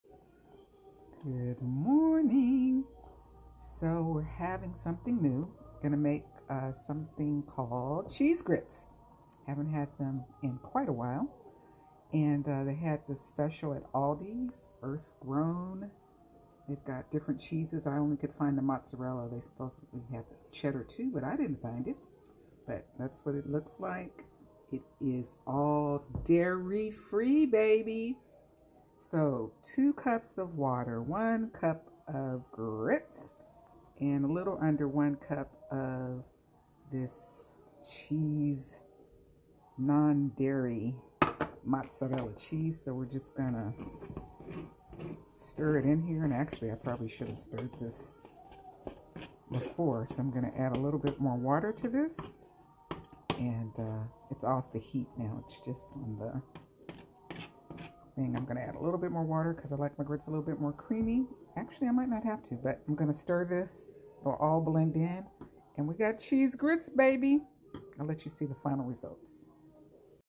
Vegan Cheese Grits
2 to 1 liquid to grits
Put grits in boiling water
stir in vegan cheese of choice- same amount as grits used.

Heat frozen peaches and add 2 shakes of cinnamon and allspice.